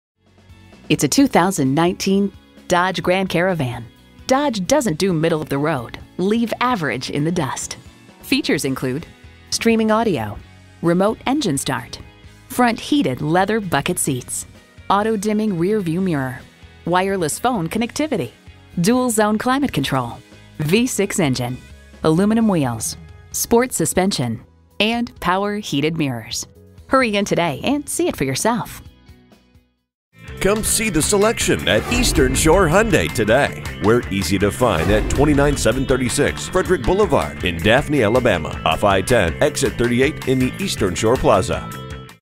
Used 2019 Dodge Grand Caravan Mobile AL Daphne, AL
Year: 2019
Make: Dodge
Model: Grand Caravan
Trim: GT
Engine: 3.6L 6-Cylinder SMPI DOHC
Transmission: 6-Speed Automatic
Color: White
Interior: black
Mileage: 26755
Stock #: T13659
VIN: 2C4RDGEG1KR513311
CARFAX One-Owner. Clean CARFAX. White 2019 Dodge Grand Caravan GT FWD 6-Speed Automatic 3.6L 6-Cylinder SMPI DOHC LIFETIME WARRANTY, LEATHER, NAVIGATION, CLEAN CARFAX, 1 OWNER, Navigation system: Garmin, Quick Order Package 29N.brbrLIFETIME WARRANTY.brbrbrLIFETIME WARRANTY.

Address:
29736 Frederick Boulevard